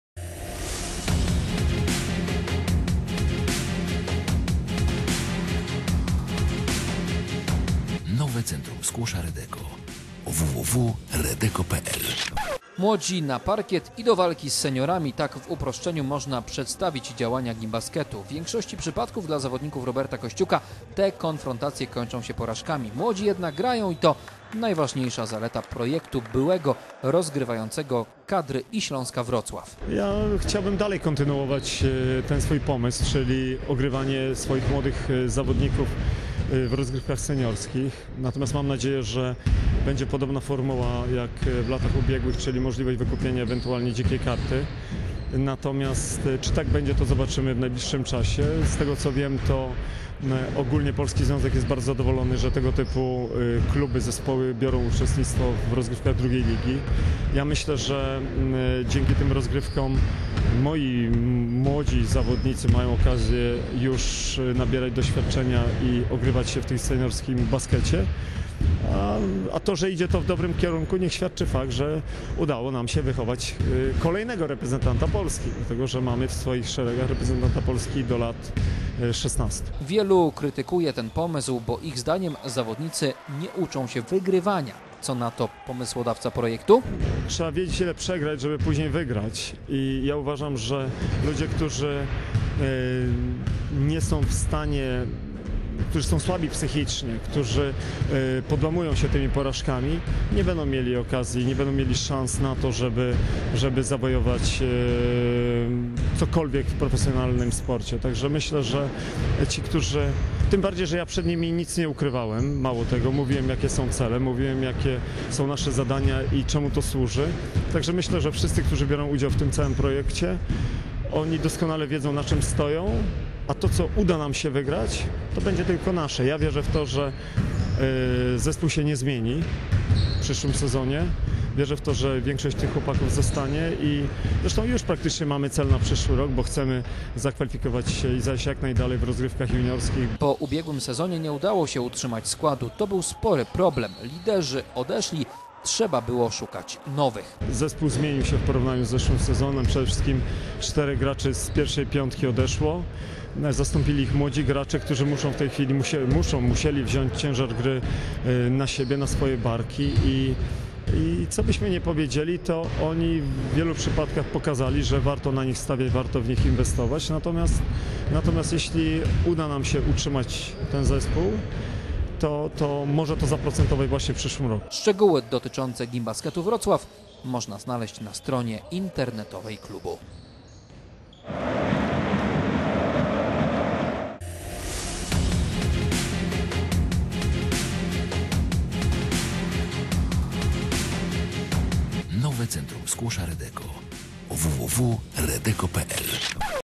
Gimbasket
Projekt kontrowersyjny, który często jest na ustach wielu koszykarskich ekspertów i takich, którzy za ekspertów się tylko mają. Mowa o drużynie drugiej ligi koszykarzy Gimbaskecie Wrocław. Zespół założony i prowadzony przez byłego reprezentanta Polski Roberta Kościuka nie wygrał w tym sezonie meczu. Mimo to Kościuk projekt zamierza kontynuować.